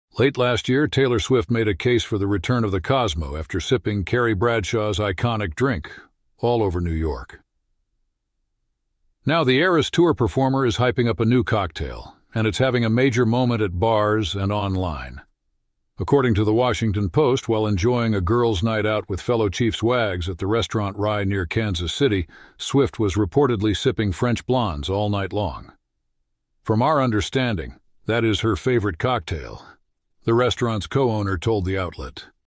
Taylor Swift's New Favorite Cocktail Should Be On Your Super Bowl Menu
Video/Image Present- From Instagram/google

Late last year, Taylor Swift made a case for the return of the Cosmo after sipping Carrie Bradshaw's iconic drink all over New York. Now, the Eras Tour performer is hyping up a new cocktail, and it's having a major moment at bars and online.